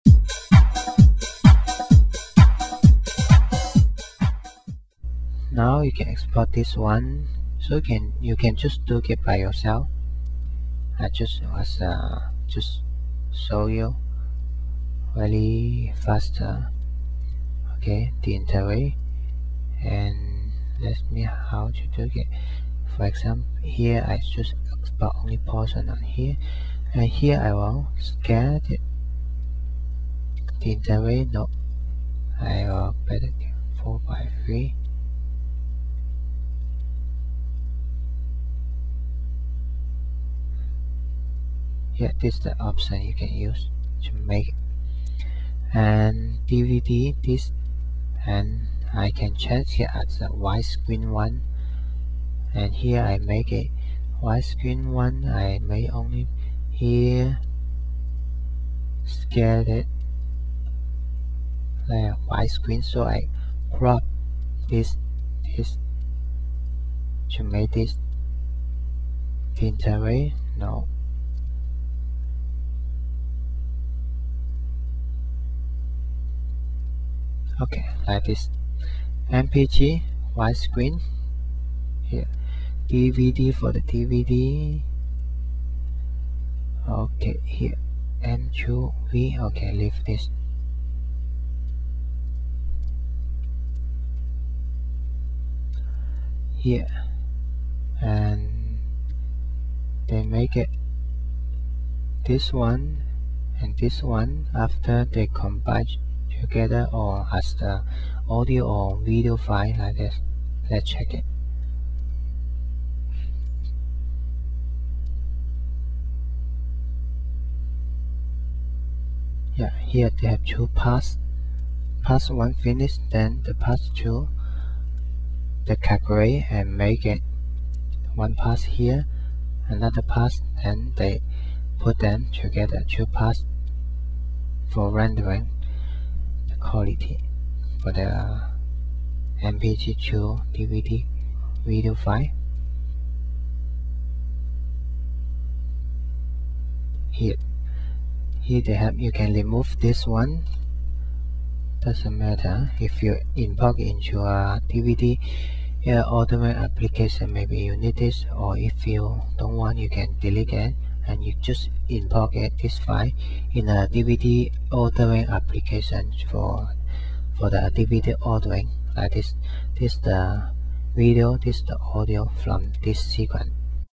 Exporting the sequence in Adobe Premiere Pro 2.0 - To a MPEG2 DVD movie files.mov
The full size and high quality trainings are available free